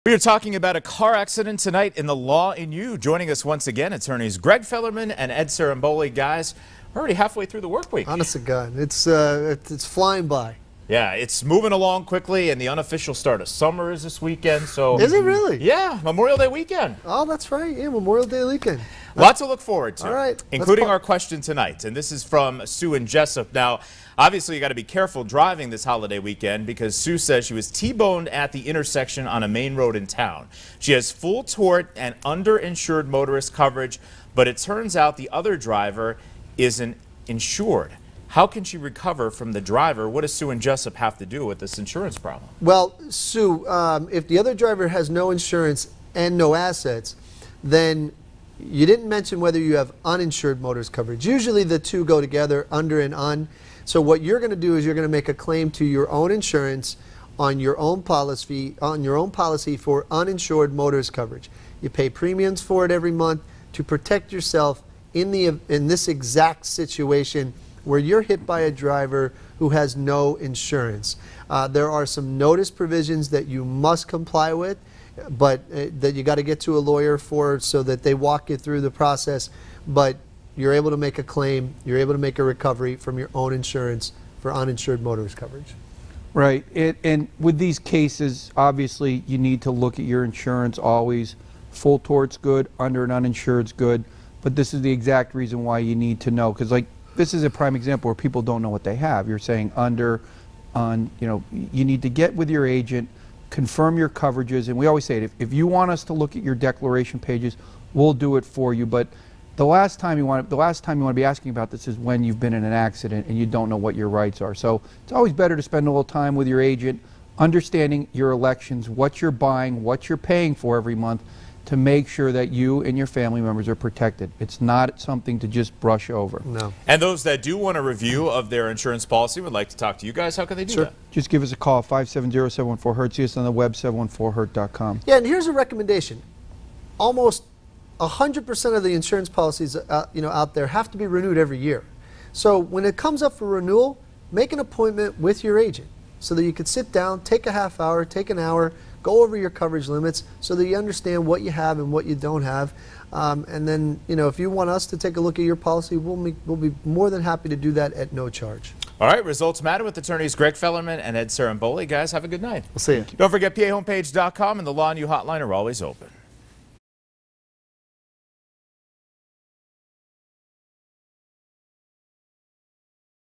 I was in a car accident and have full tort, the driver is uninsured, how am I going to recover?
A woman was T-boned and the driver is uninsured. She has full tort and underinsured motorist coverage. How is she going to recover from the other driver? Find out on this Law & You with attorneys Fellerman and Ciarimboli.